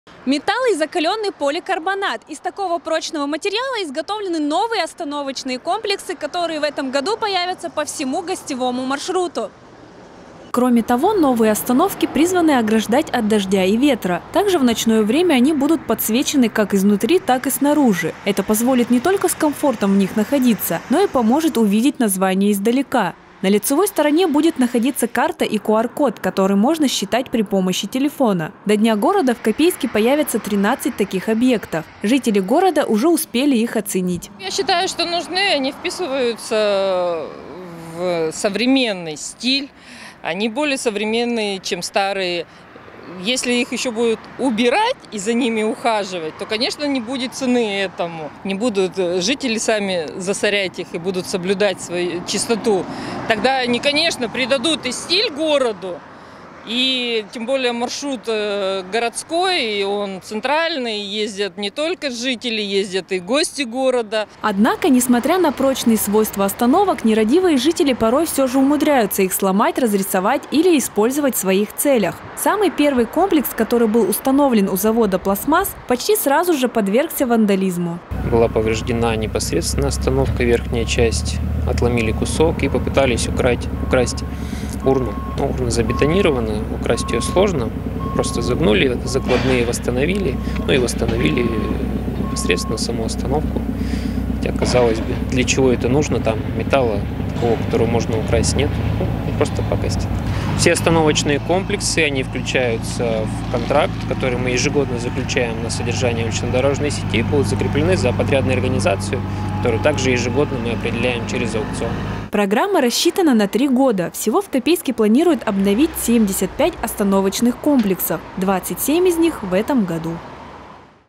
В Копейске приступили к установке остановок
Новые и прочные. 27 новых остановочных комплексов появятся в Копейске в этом году. Подрядчик уже приступил к их монтажу.